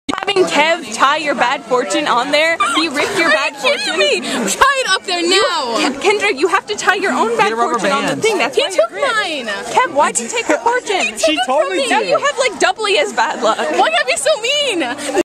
Bad Luck, Part 1
When you receive a bad fortune (omikuji) in Japan, you're supposed to tie it at the temple so it doesn't follow you when you leave!